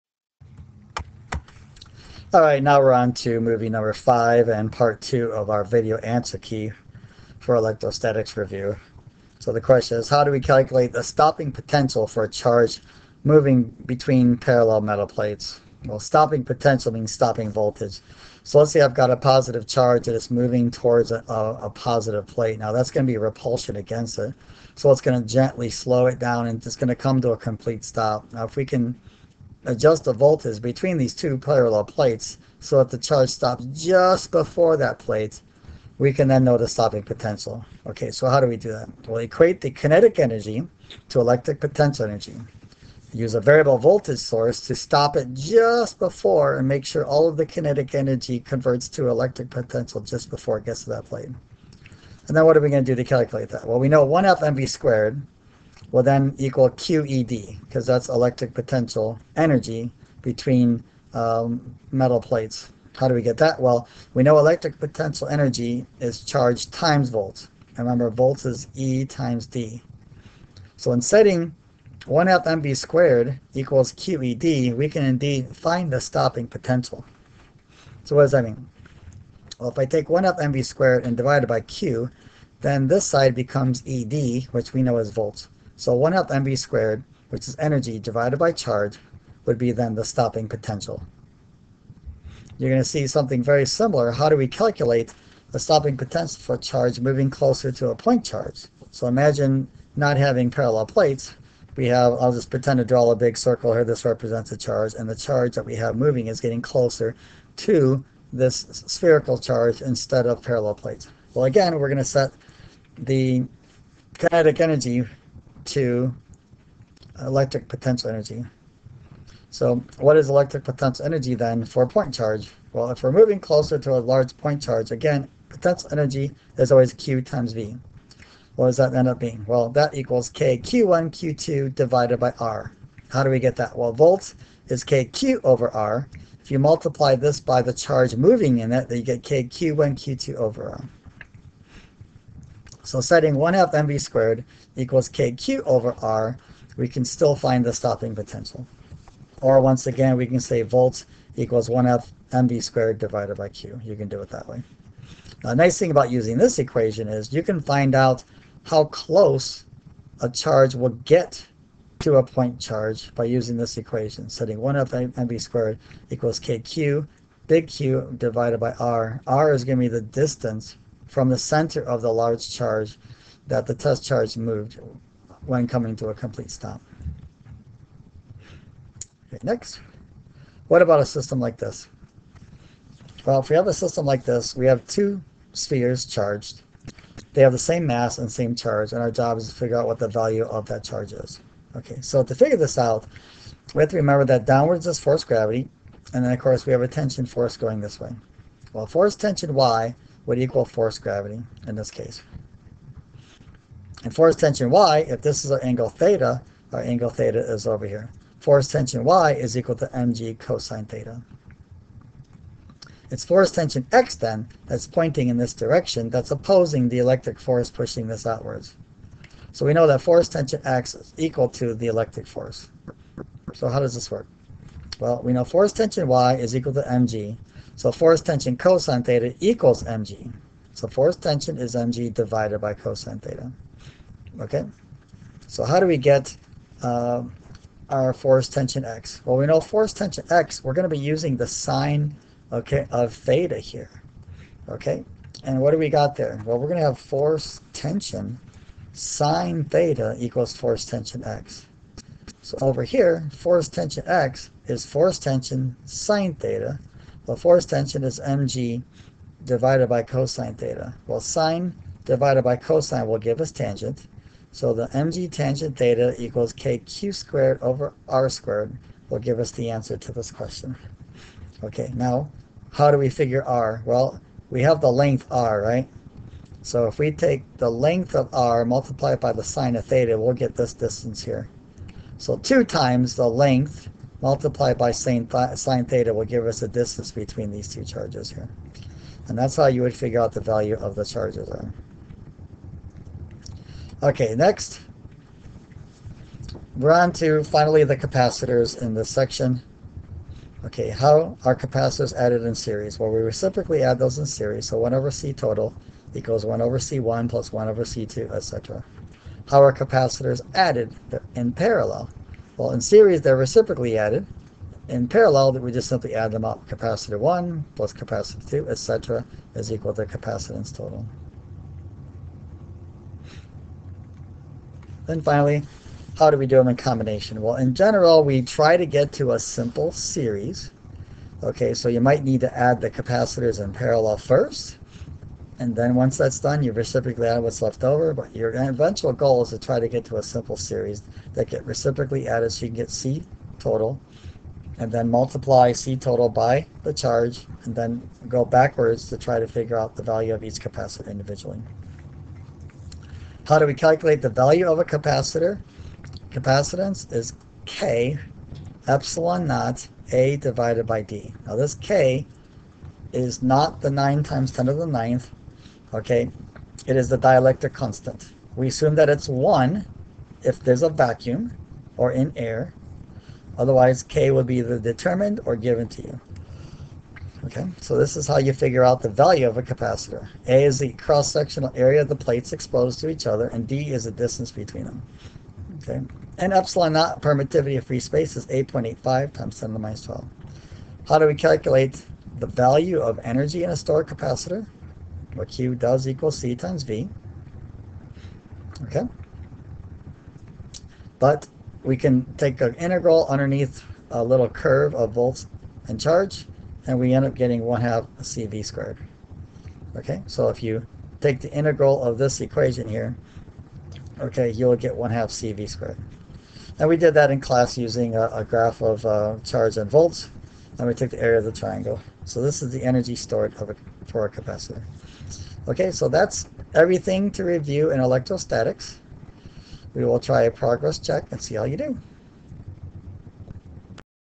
Review 2 for Electrostatics for A.P. Physics 2
This is the second video for the answer key for electrostatics concepts for A.P. Physics 2